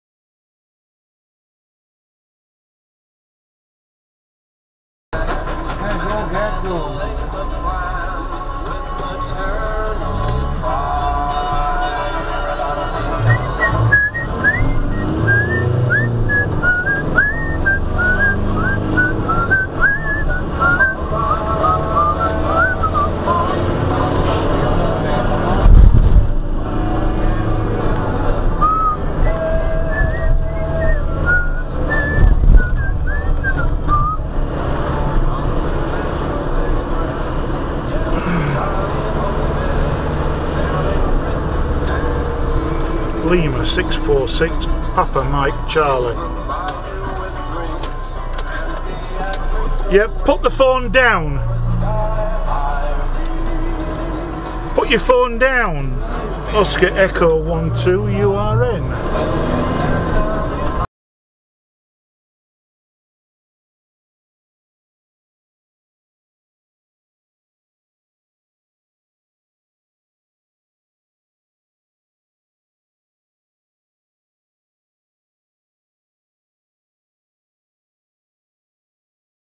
OE12 URN Mobile in right hand, Apple in left!
Initially, I was looking at the car in the right hand lane - seems to have some sort of problem deciding which lane he wants to be in.
THEN, my way is blocked by a guy who is trying to drive his car whilst holding a mobile to his ear with his right hand AND eating an apple he is holding in his left hand!
I cannot get through until the car behind moves out of the way.